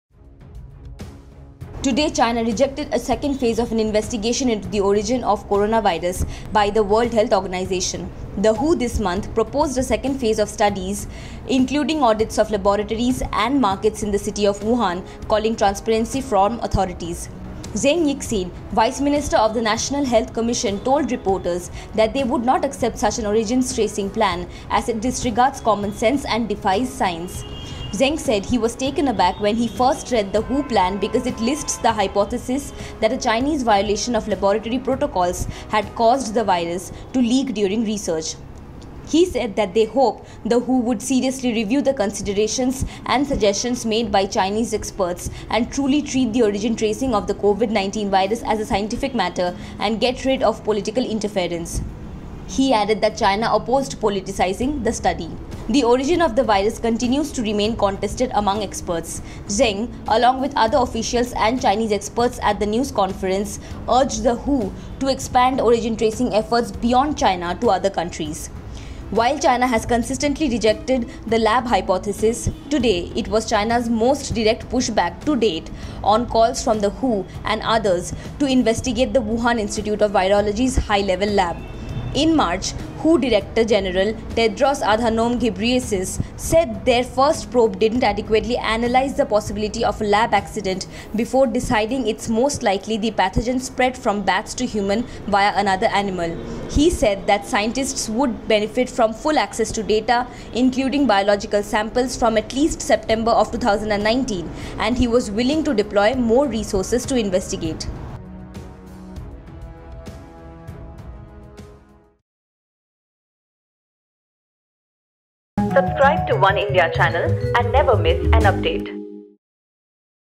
China opposes WHO's plan for a second phase of investigation on the origins of Covid | Oneindia News
Today, China rejected the second phase of an investigation into the origin of the coronavirus by the World Health Organization. The WHO this month proposed a second phase of studies into the origins of the coronavirus in China.